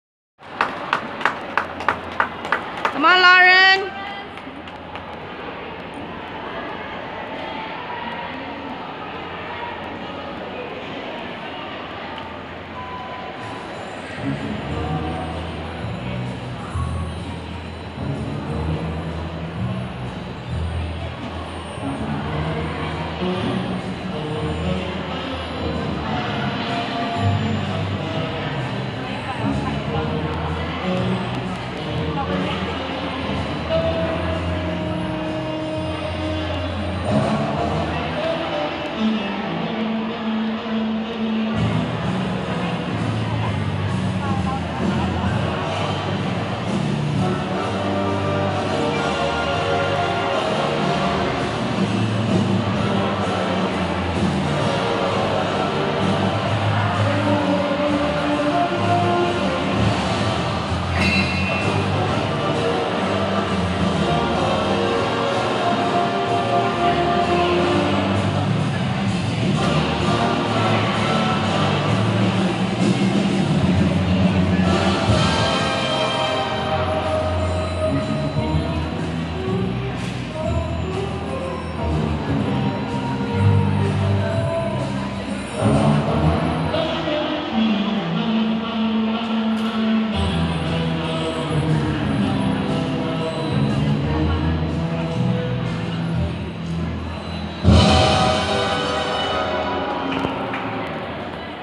Rhythmic Gymnastics Lauren's ball routine 16th August 2016 Karnival Bakat Pelapis Negeri Siri 2
Lauren's ball routine in her first rhythmic gymnastics competition in Penang, Malaysia. She scored a 7.25 and finished in 8th place overall for the ball routine. She finished 5th overall through 3 routines and Pahang state came in 3rd place